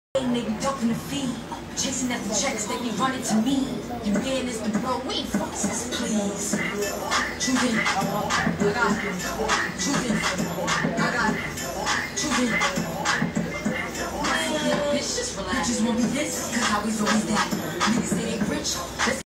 Richard Leonard on the color and install, bundles from Precise Hair (they took bleach really
Richard Leonard on the color and install, bundles from Precise Hair (they took bleach really well) if your in Atl tap in with chocolate and Precise will ship bundles to wherever you are ❤️👌🏼